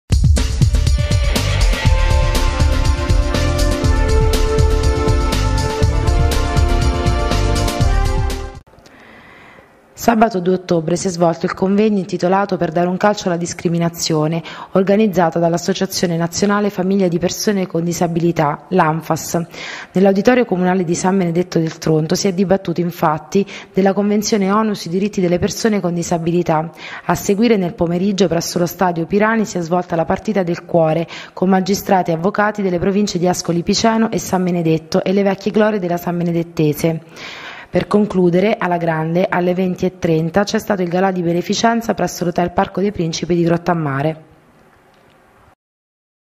Convegno Anfass
Si è svolto sabato 2 ottobre il convegno per dare un "Calcio alla discriminazione" organizzato dall'Anfass e incentrato sui diritti delle persone con disabilità.